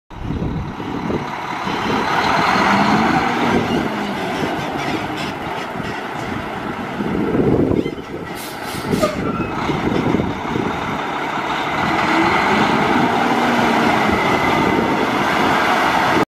2017 VOLVO VNL62T400 For Sale
2017 VOLVO w/ NEW LOADLINE 22’ GRAIN BOX WITH AUTOMATED TRANSMISSION
Volvo Engine, 425 HP, Rear Pusher, Beat/Silage Equipped, Roll Tarp, Rear Controls, New Build, Tilt, Cruise, AC, New Virgin Rubber, Call Ask For Jim. More Photos Coming Soon.